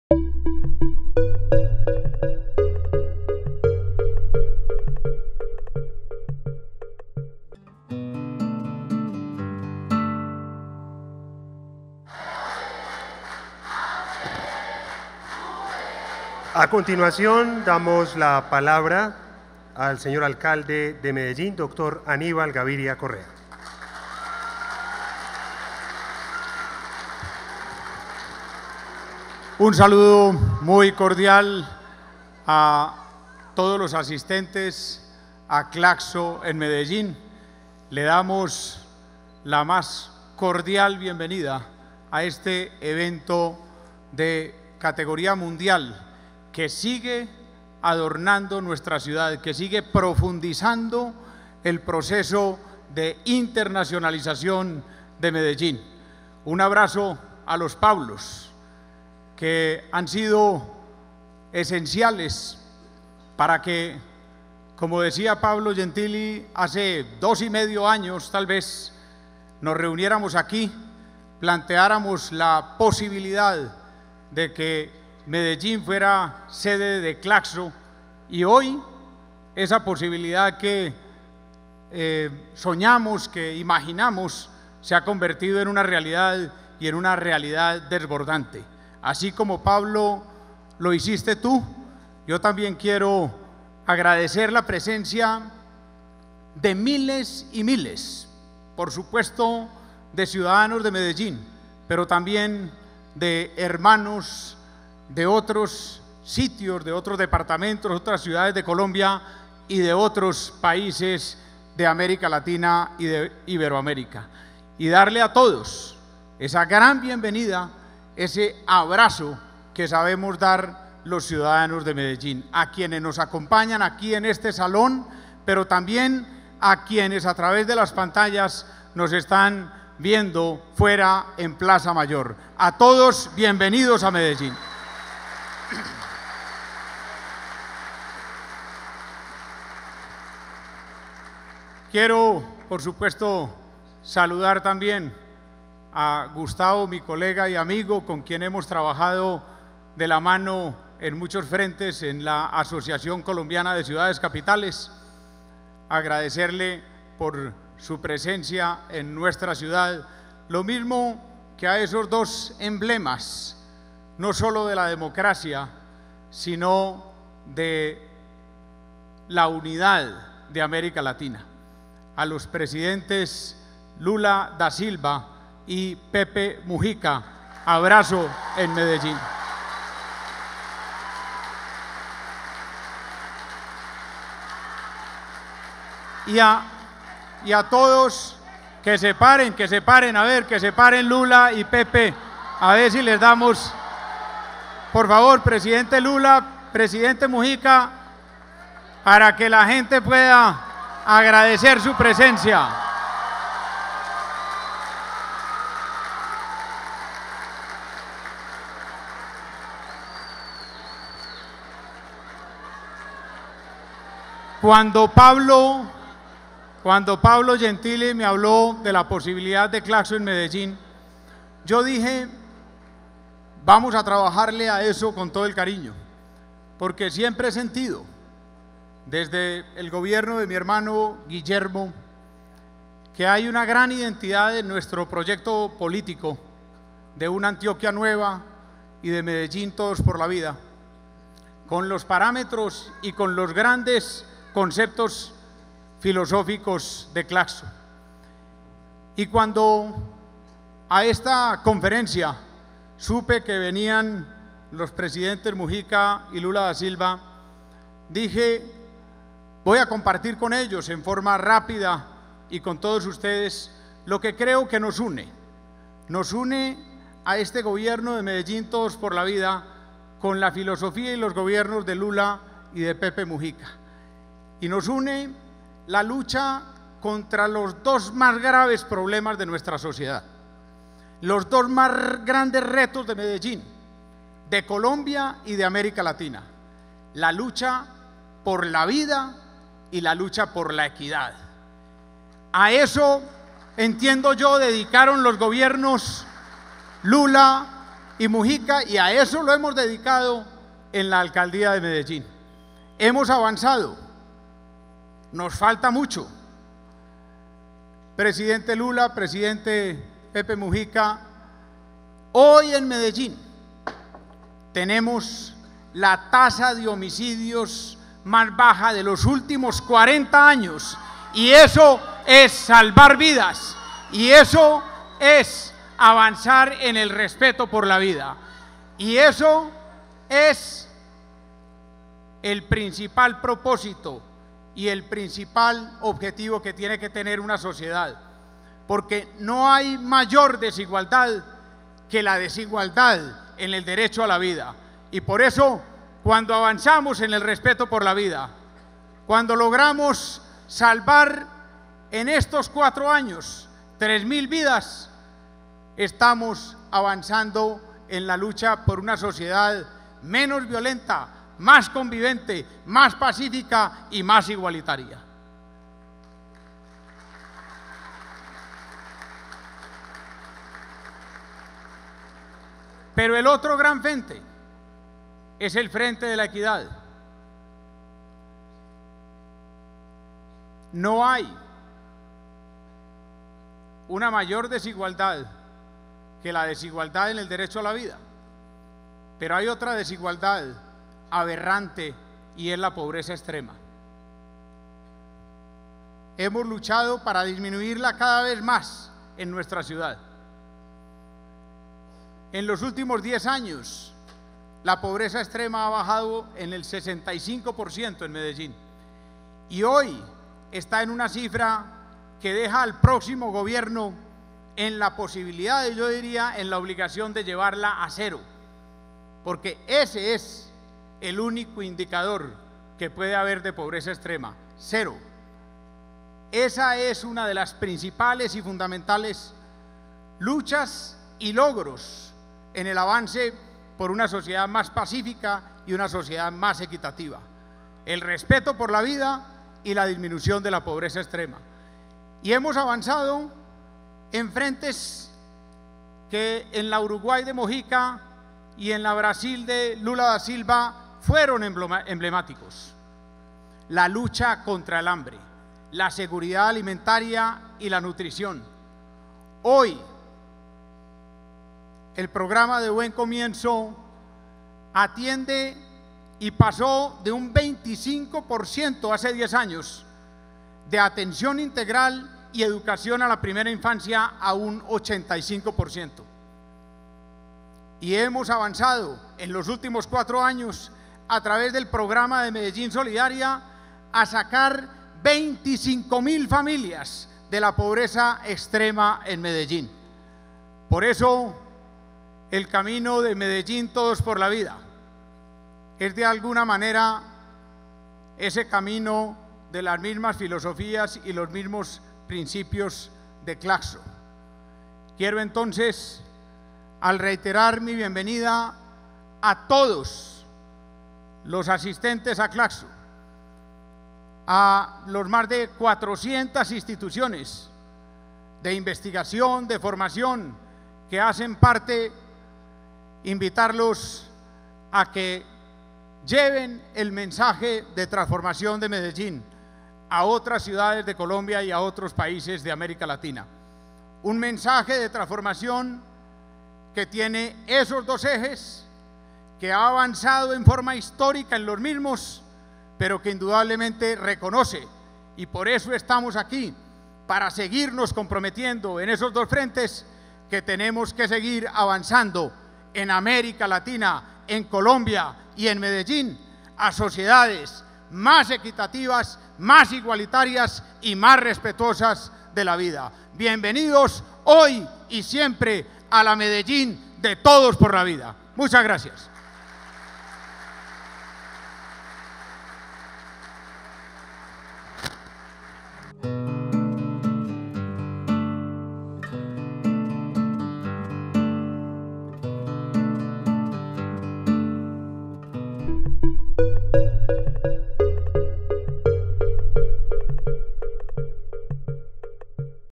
Anibal Gabiria en la apertura de la VII Conferencia CLACSO2015 - Parte 4
Acto de apertura de la VII Conferencia Latinoamericana y Caribeña de Ciencias Sociales.

Anibal Gabiria,  Alcalde de la ciudad de Medellín.

VII Conferencia Latinoamericana y Caribeña de Ciencias Sociales "Transformaciones democráticas, justicia social y procesos de paz", del 9 al 13 de noviembre de 2015 en Medellín, Colombia.